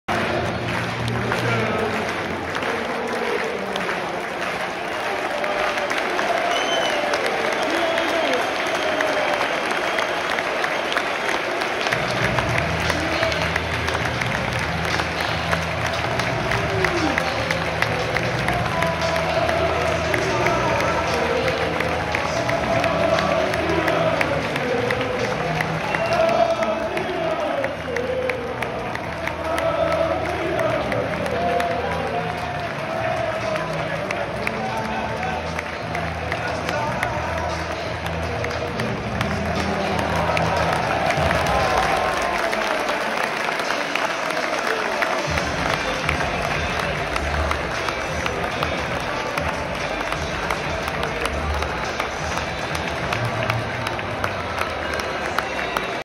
Chelsea 7-0 Norwich (Premier League 2021/2022)
Goals by Mount (8’, 85’, 90+1’), Hudson-Odoi (18’), Reece James (42’), Chilwell (57’), Aarons OG (62’)
Stamford Bridge – London, England